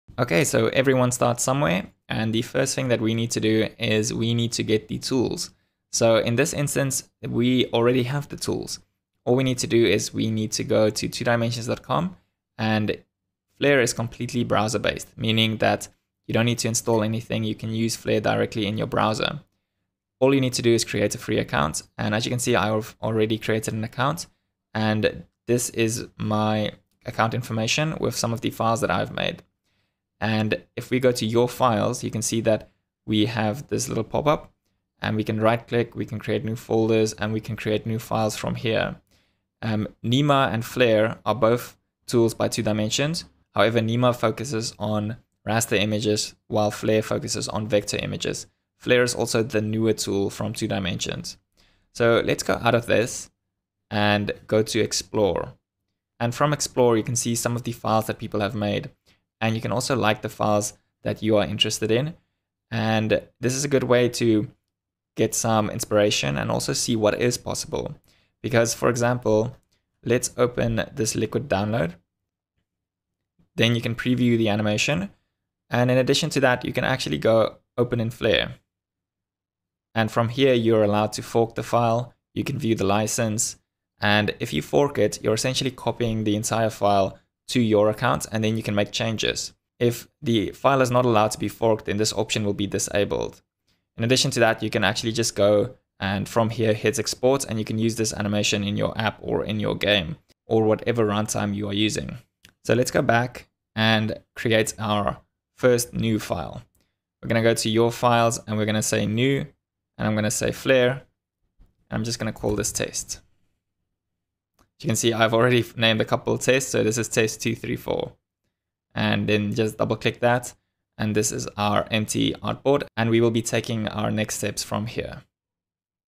Rive: Vector Animation - #02 Getting Started
Some important concepts and techniques to create smooth looking animations and loops.

Course:
Learn to use the Flare Animation tool for vector animation to create great looking animations for your games and apps.